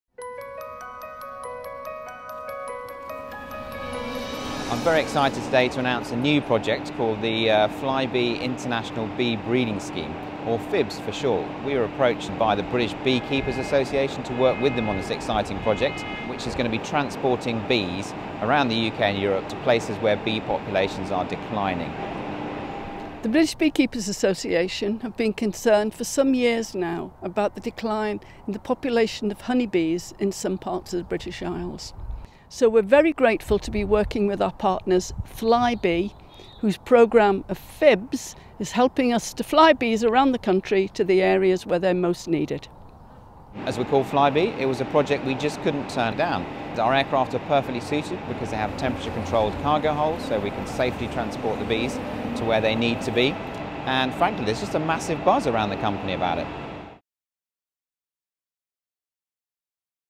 The Flybe International Bee Breeding Scheme (F.I.B.B.S.)
This morning, we are pleased to announce the Flybe International Bee Breeding Scheme (F.I.B.B.S.), a new partnership between Flybe and the British Beekeepers Association.